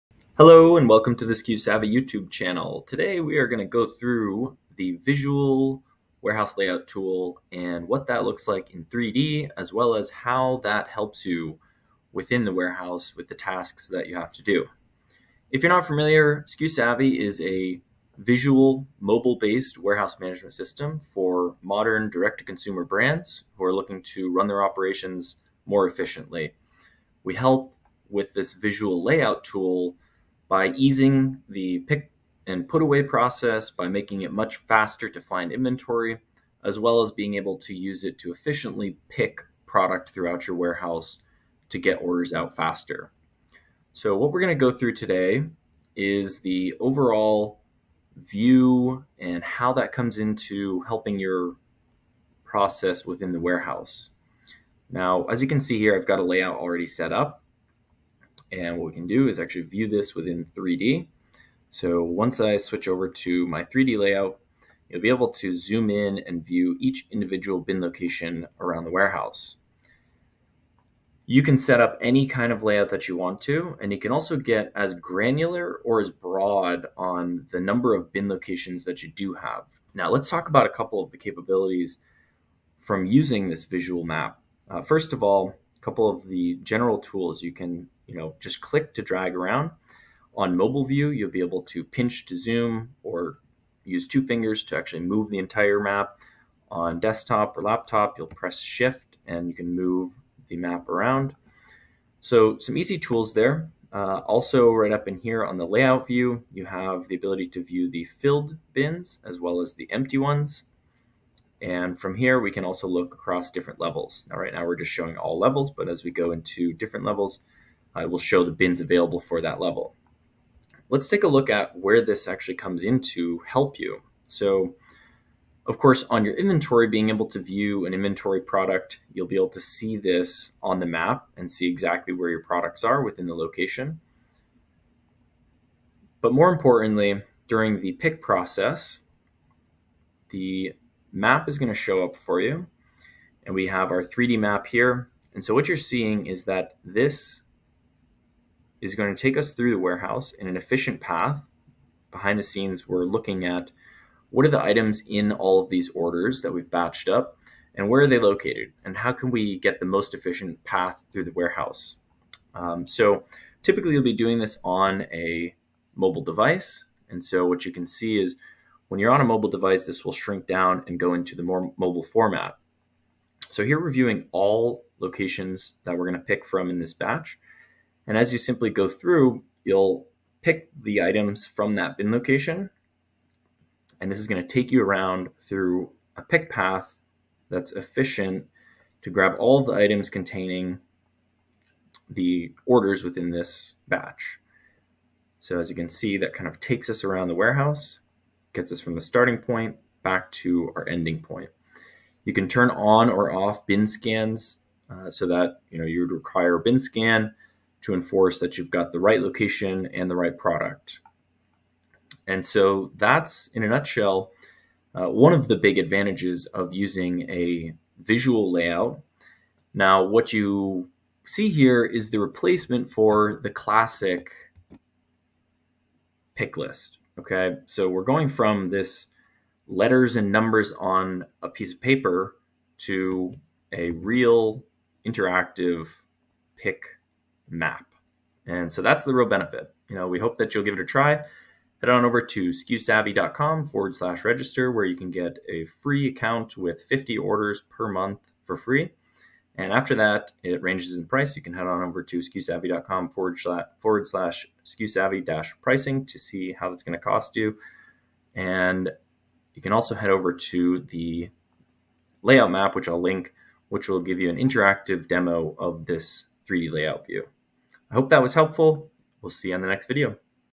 Efficient Picking in the Warehouse Using a Visual 3D Warehouse Layout
How can you most efficiently map the fastest path through your warehouse to pick 10 orders?

In this video we'll go through a visual pick path as a method of figuring outing the fastest way to pick product from your warehouse.